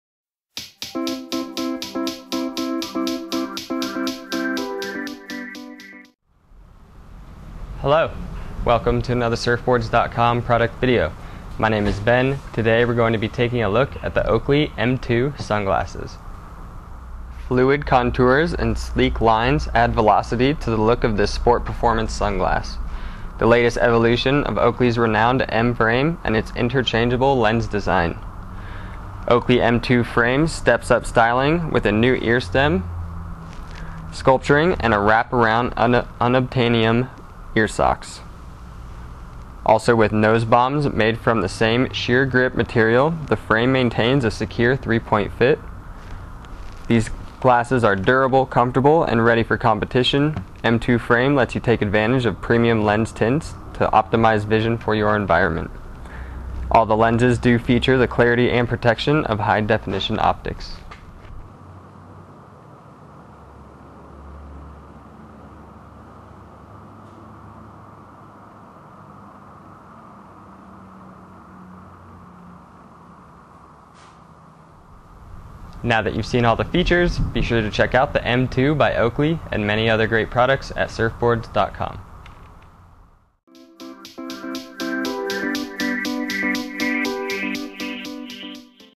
A Closer Look At The Oakley M2 Sunglasses
Fluid contours and sleek lines add velocity to the look of this sport performance sunglass, the latest evolution of Oakley's renowned M Frame and its interchangeable lens design. Oakley M2 Frame steps up styling with new earstem sculpturing and wrap-around Unobtainium earsocks, and with nosebombs made of the same sure-grip material the frame maintains a secure Three-Point Fit. Durable, comfortable and ready for competition, M2 Frame lets you take advantage of premium lens tints to optimize vision for the environment, and all lenses feature the clarity and protection of High Definition Optics (HDO).